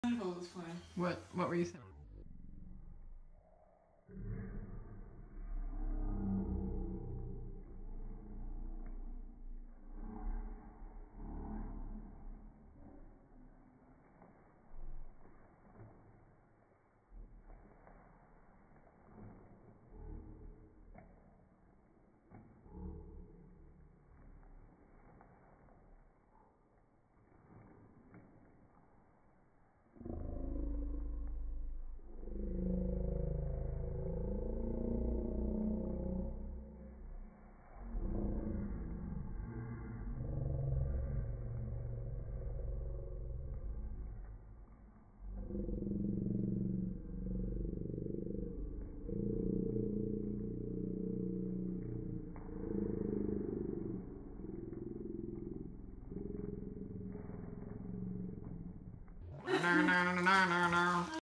Unpacking the tree
Katr accidentally made a slo mo video of me pretending the Christmas tree box is a wang and I'm posting it on here because Typepad won't let you upload videos to their platform.